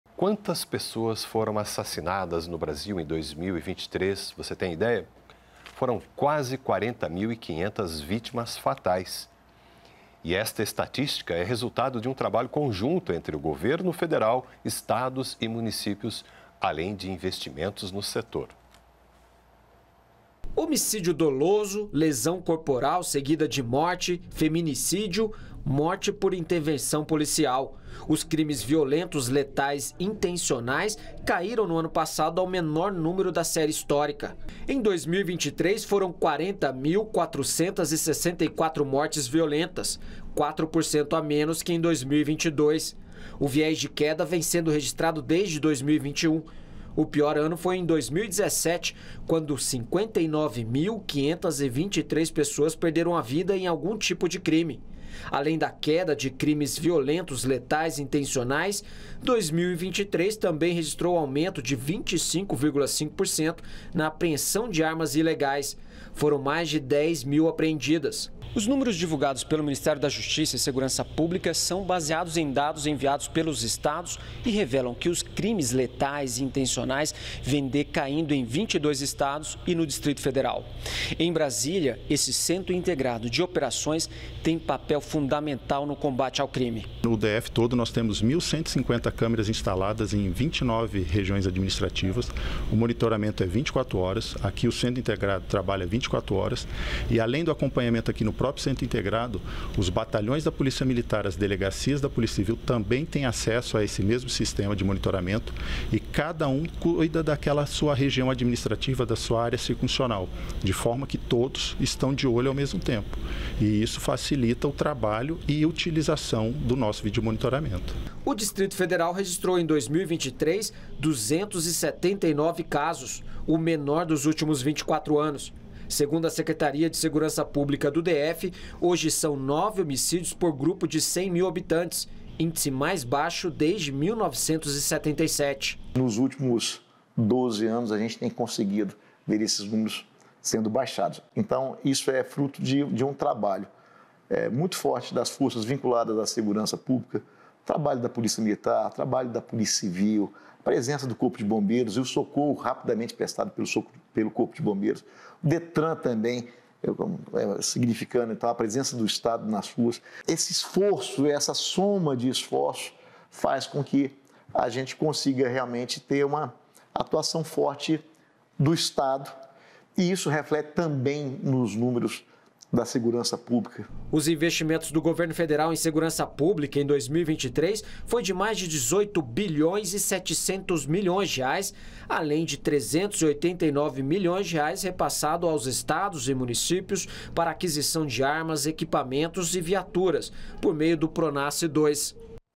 CRIMES VIOLENTOS LETAIS INTENCIONAIS CAEM AO MENOR NÚMERO DA SÉRIE HISTÓRICA EM 2023
Os crimes violentos letais intencionais caíram ao menor número da série histórica em 2023. A redução é resultado de trabalho conjunto entre os governos federal, estaduais e municípios, além do maior investimento em segurança pública.